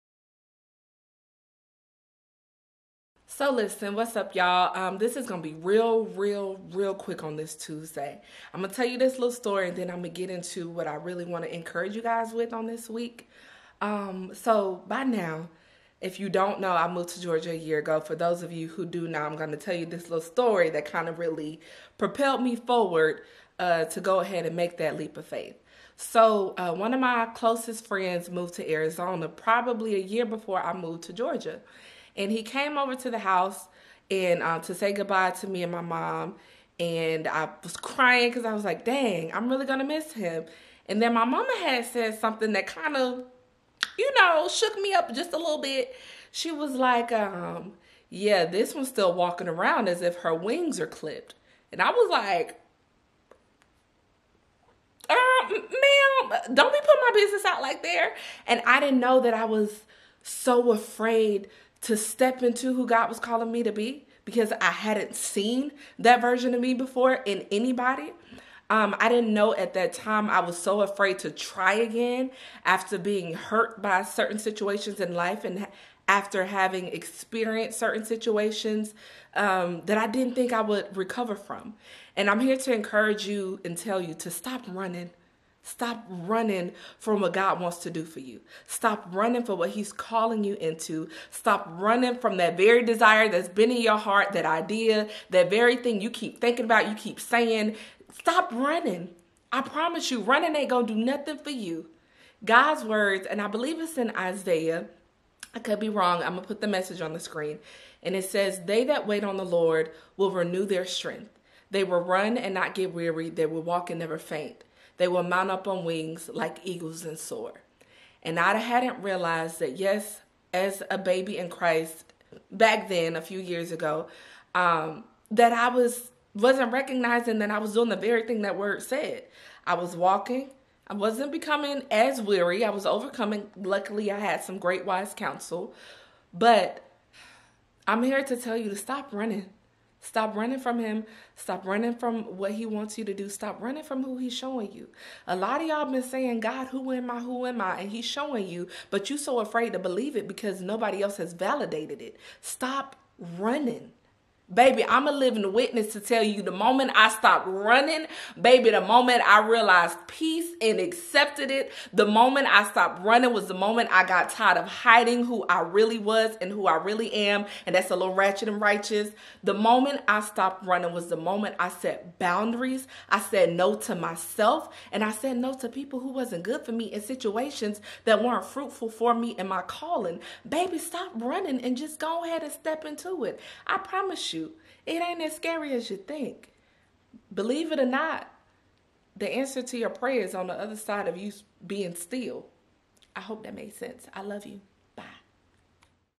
STOP RUNNING
So listen, we often pray for things but run from the answer. Let this message be a gentle reminder to stop running and embrace the answer that is in front of you.

**LOCK IN WITH LAY
Join me throughout this vlogging journey as I turn my Fashion Designing dreams into reality and encourage others along the way.